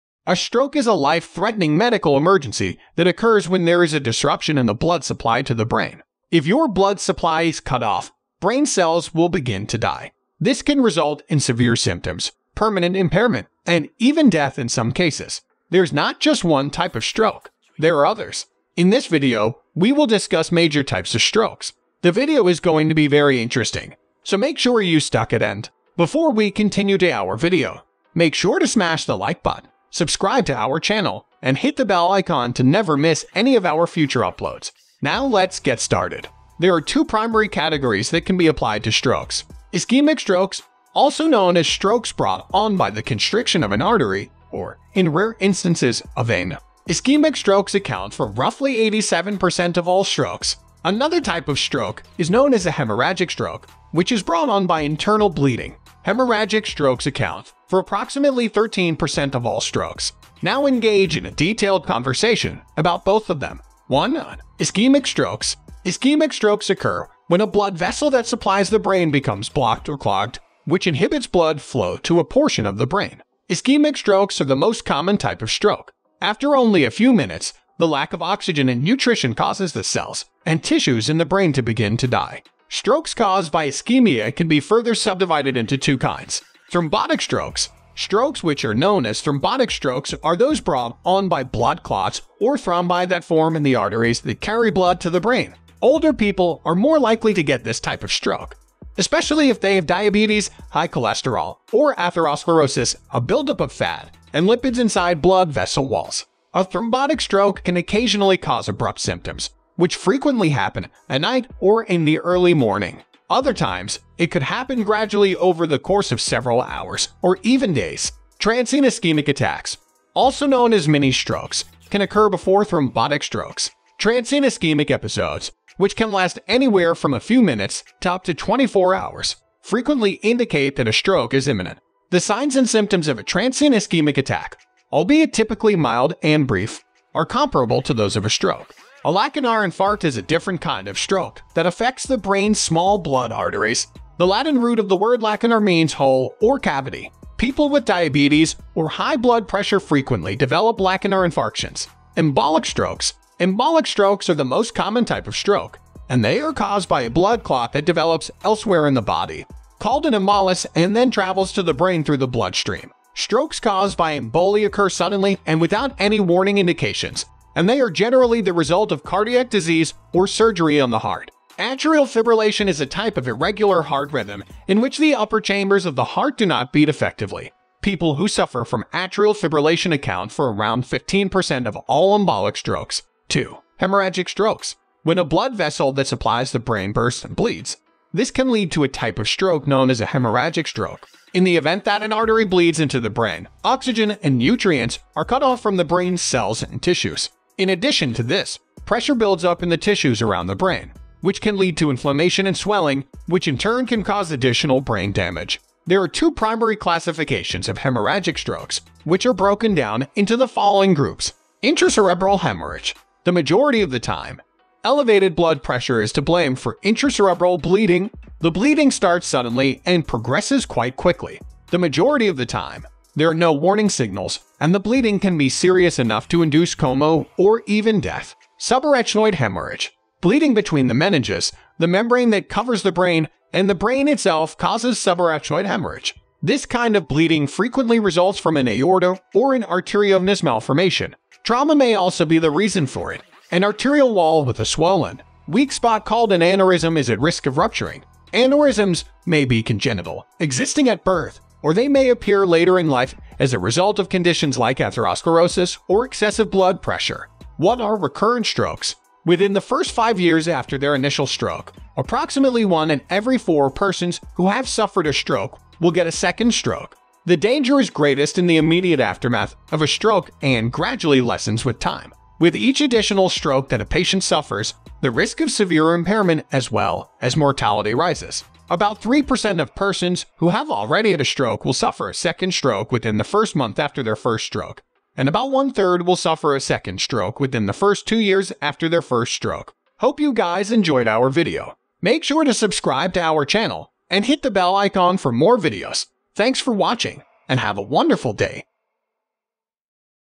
The Different Types of Stroke
Advanced information to a health condition called Stroke. What are the various different types of strokes? How many do you know and understand? What are the different signs and symptoms these individual types show?

Understand the causes and consequences of Stroke to better prevent it early. If you know someone who has the condition or might have it in the long run, gain the knowledge and skills to give a helping hand and show your it!ost concern.

Tips to make your life and health easier and fulfilling.